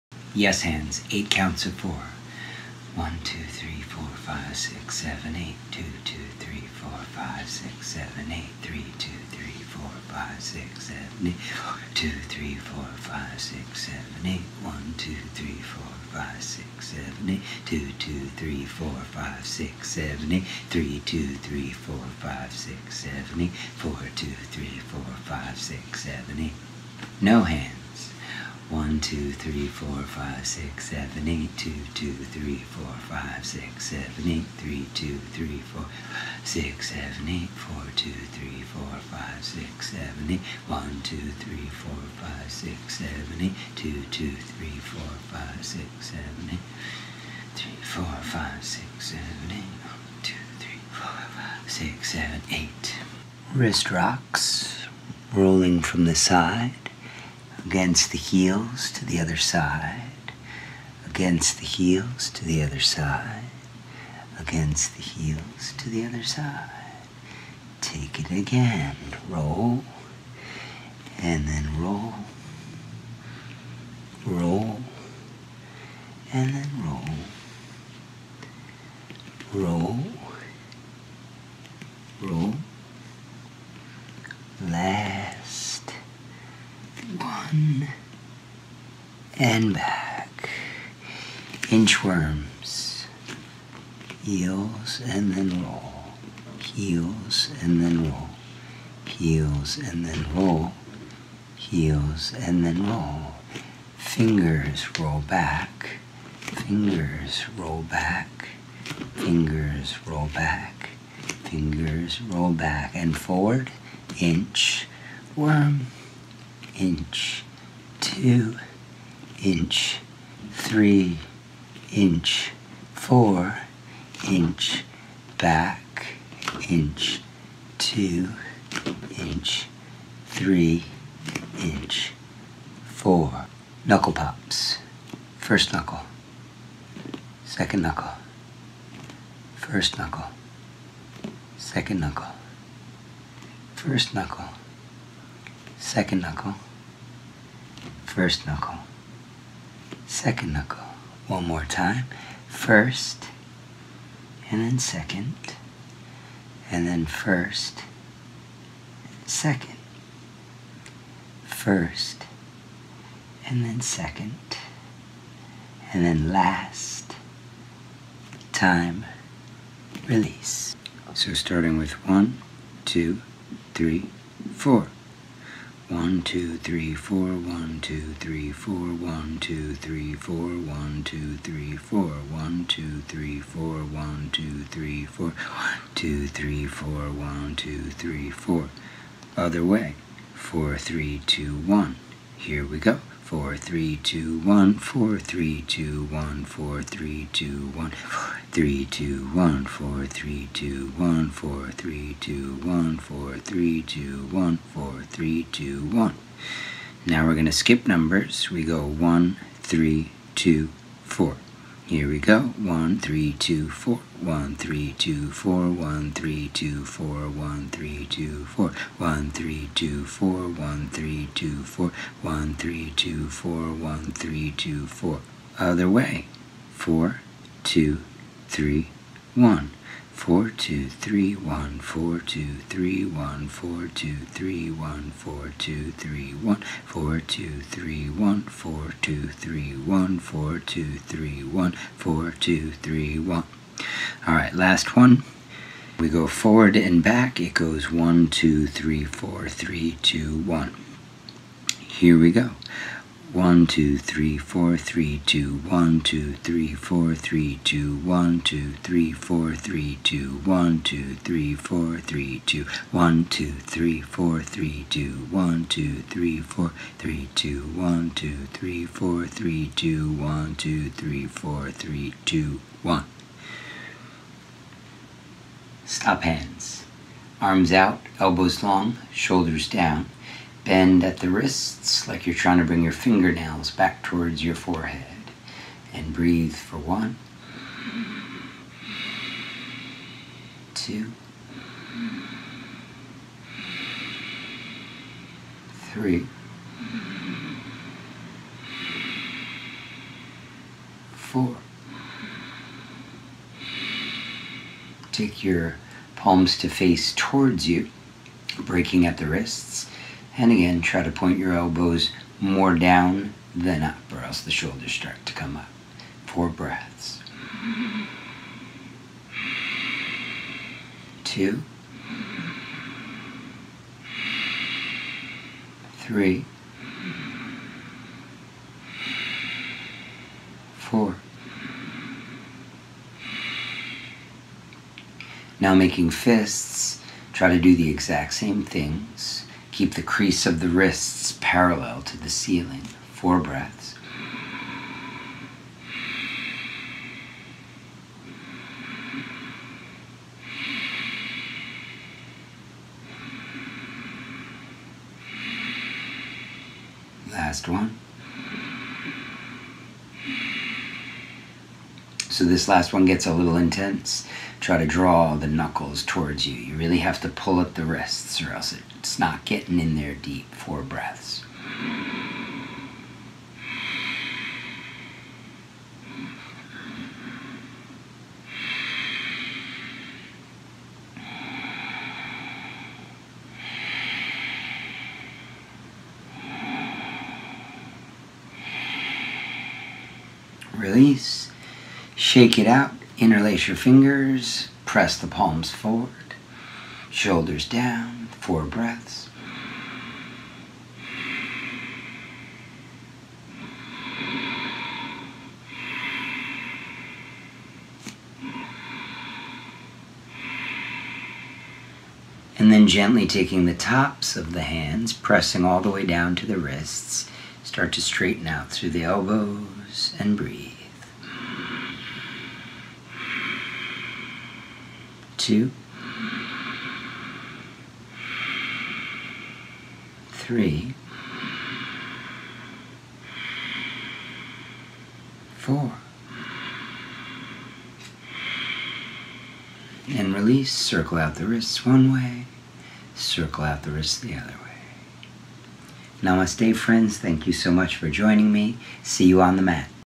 Warm up the wrists warm up the brain!
This is part of my every practice warm up. It helped a long time wrist injury back to health.